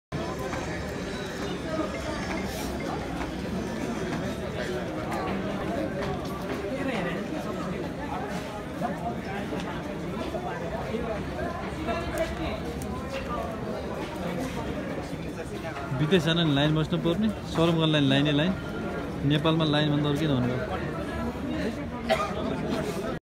Saram office Maa line Nepal
Hame Nepali Bides Jada ne Garo , paisa Kamai aai saram garda ne Ghantau line Maa basna parne pida x hjur Nepal Maa